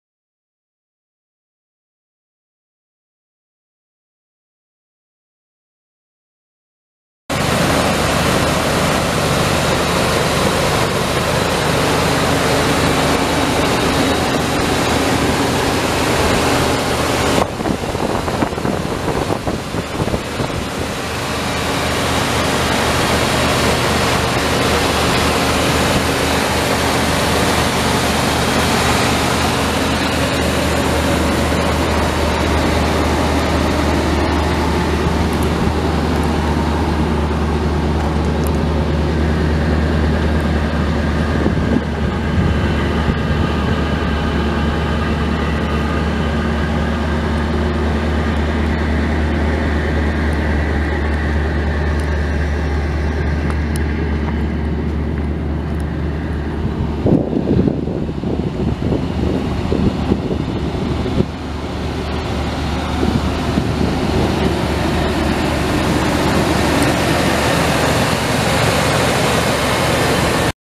1990 Chevrolet Suburban R1500 SUV for sale | no-reserve Internet auction April 3, 2018
Government Auction
View a detailed listing of this item and place bids

1990 Chevrolet Suburban R1500 SUV
205,234 miles on odometer
5.7L V8 OHV 16V gas engine
Automatic transmission
AC and heat
Tilt
Cruise
AM/FM
Vinyl interior
P235/75R15 tires
Receiver hitch not included
Unit# 103
Kansas title